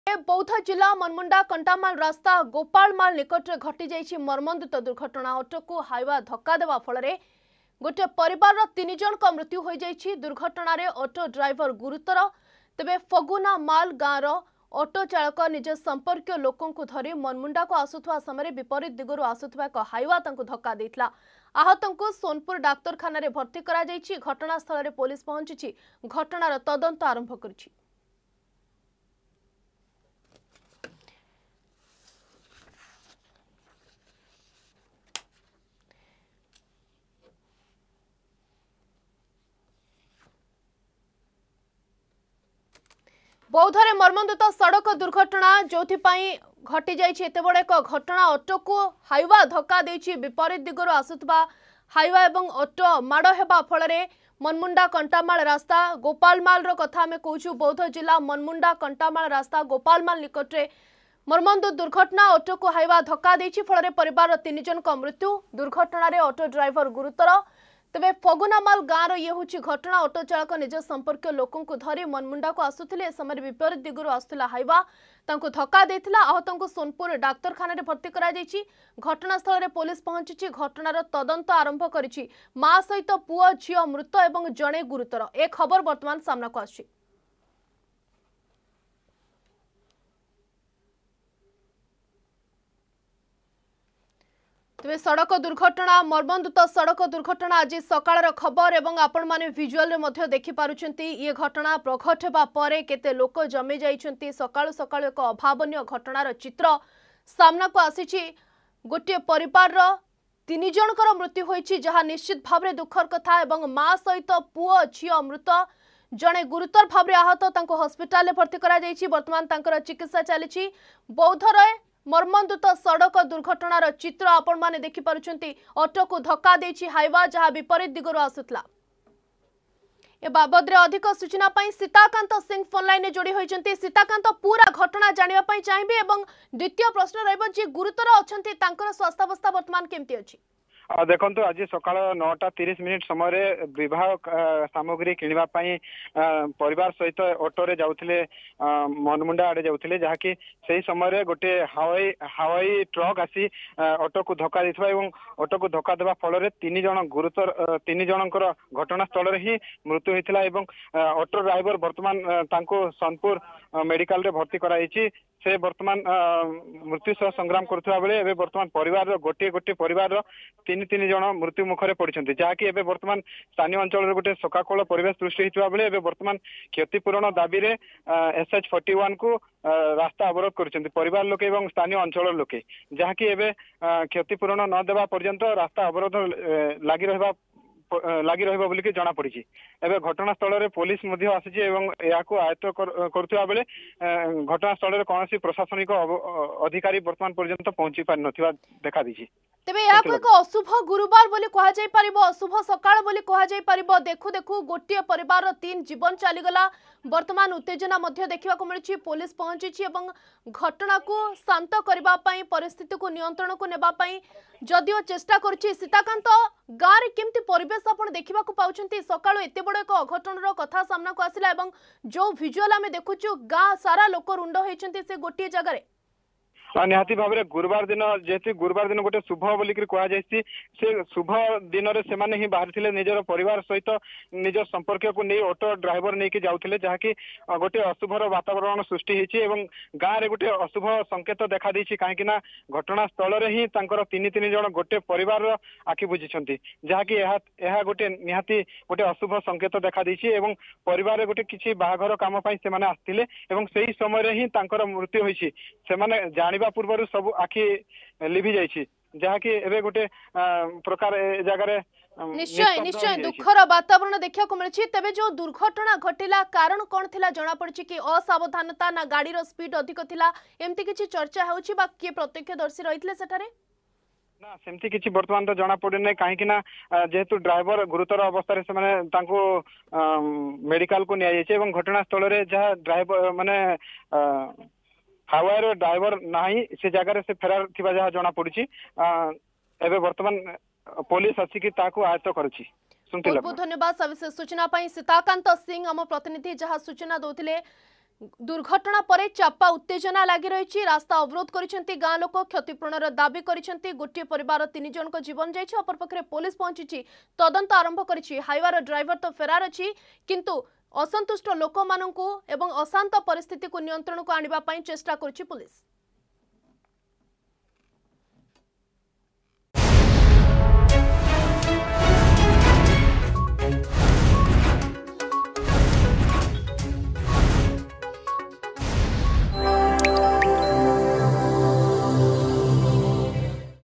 ବୌଦ୍ଧ ଜିଲ୍ଲା ମନମୁଣ୍ଡା-କଣ୍ଟାମାଳ ରାସ୍ତା ଗୋପାଳମାଲ ନିକଟରେ ମର୍ମନ୍ତୁଦ ଦୁର୍ଘଟଣା ....
ବୌଦ୍ଧ ଜିଲ୍ଲା ମନମୁଣ୍ଡା-କଣ୍ଟାମାଳ ରାସ୍ତା ଗୋପାଳମାଲ ନିକଟରେ ମର୍ମନ୍ତୁଦ ଦୁର୍ଘଟଣା । ଅଟୋକୁ ହାଇୱା ଧକ୍କା ଦେବା ଫଳରେ ପରିବାରର 3 ଜଣଙ୍କ ମୃତ୍ୟୁ ହୋଇଛି । ଦୁର୍ଘଟଣାରେ ଅଟୋ ଡ୍ରାଇଭର ଗୁରୁତର ହୋଇଛନ୍ତି .